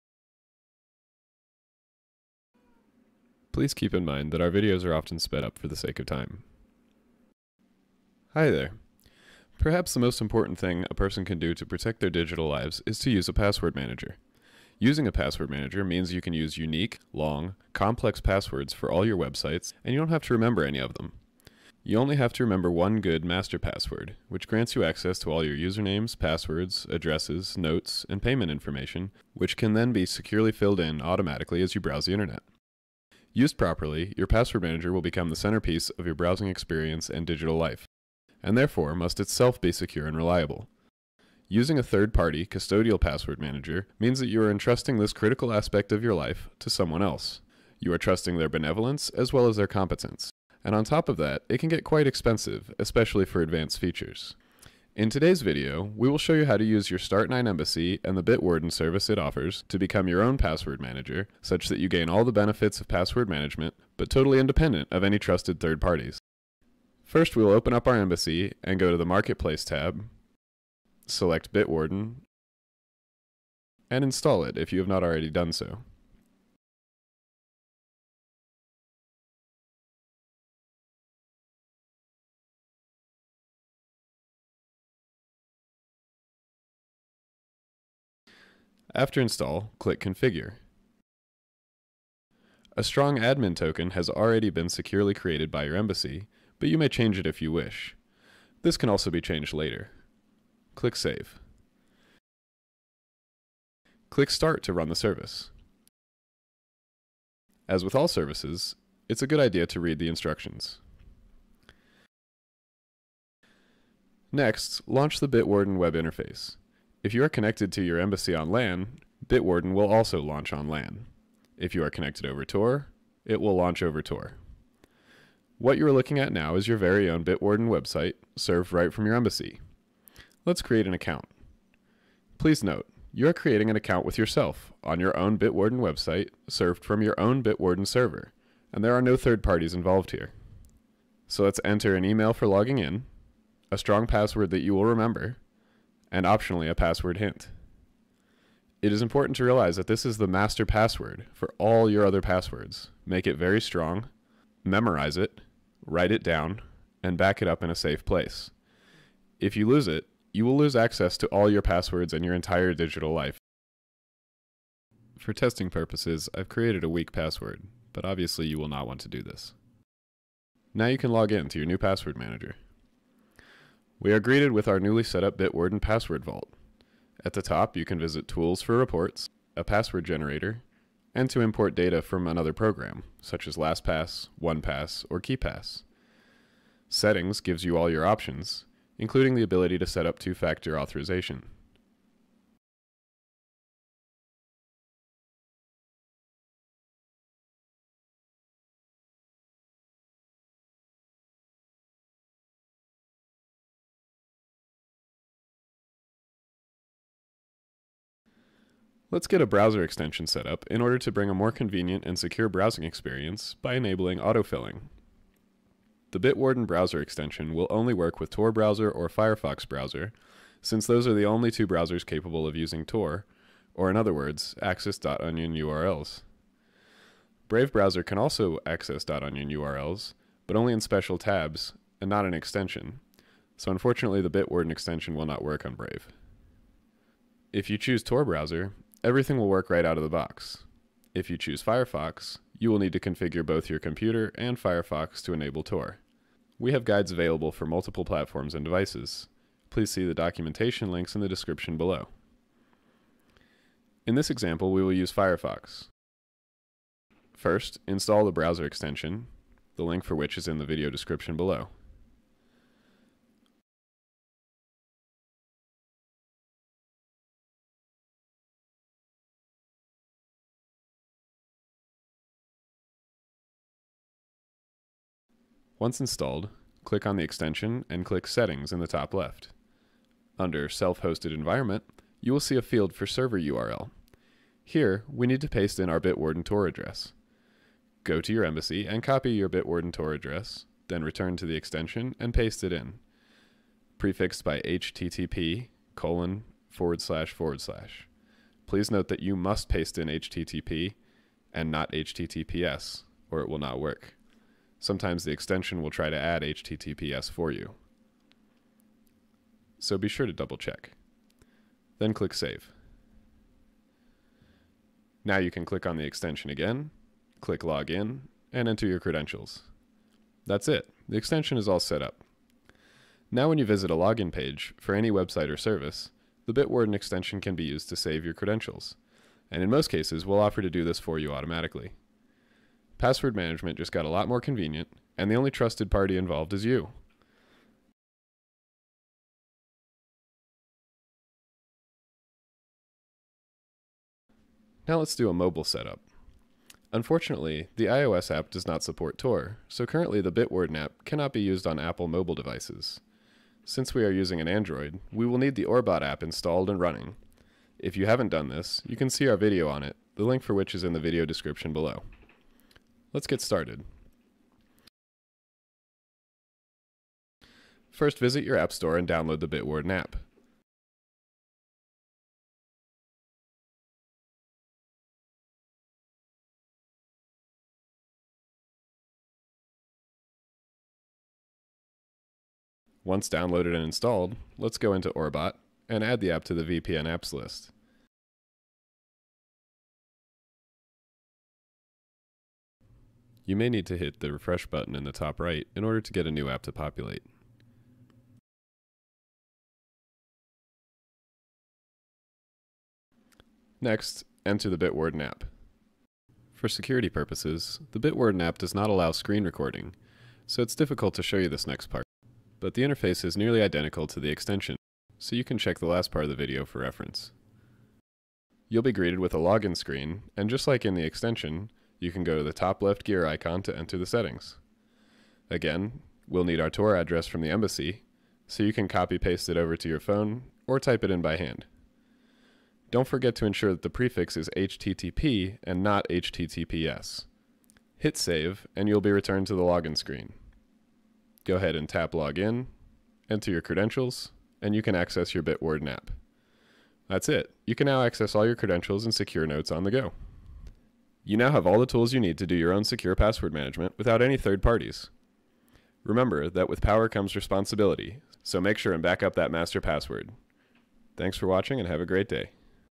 Using Vaultwarden on StartOS
In this video, we'll be going over the importance of using a password manager, and how to use Valutwarden (aka Bitwarden) on StartOS